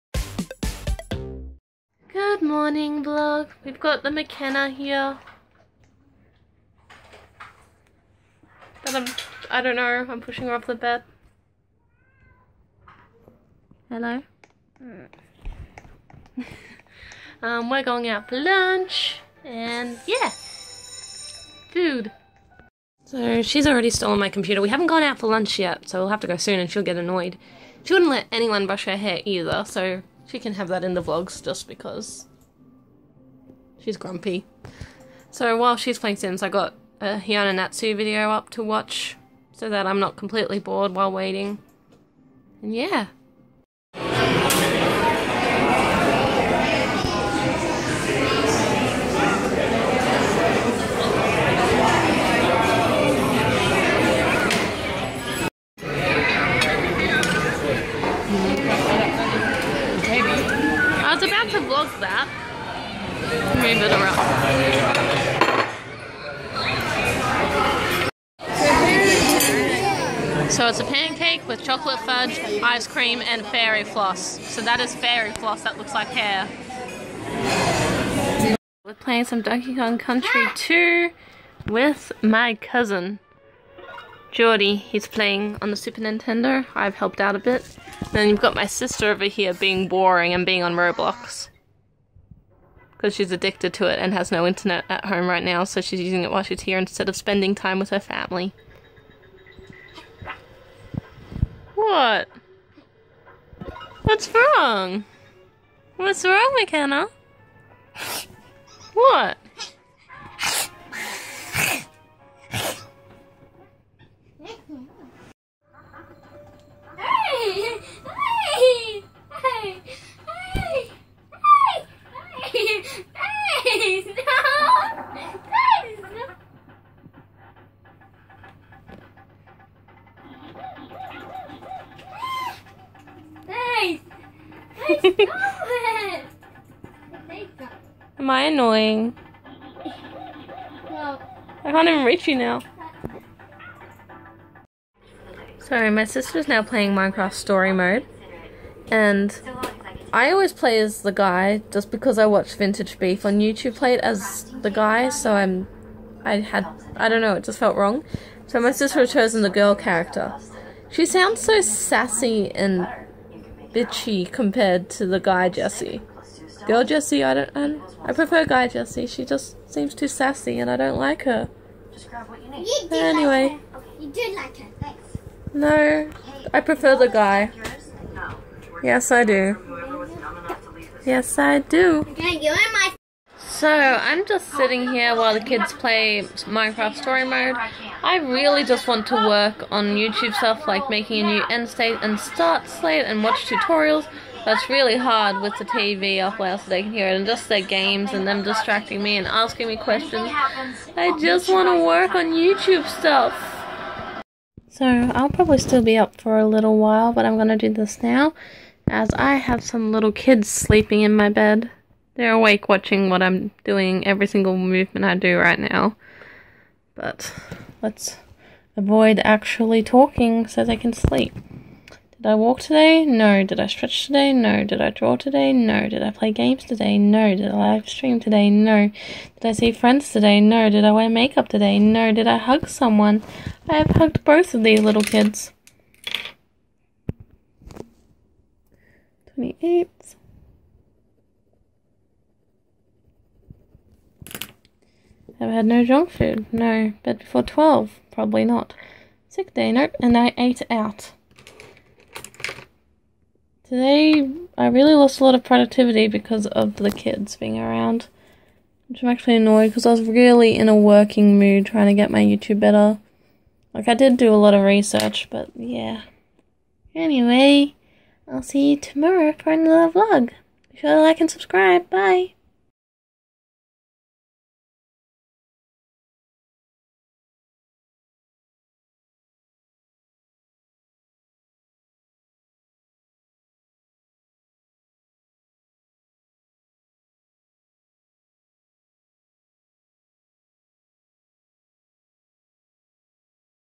Lunch and kids- Day 27
Today was a day of going out for lunch with the family then spending the day with the kids, my little sister and one of my little cousins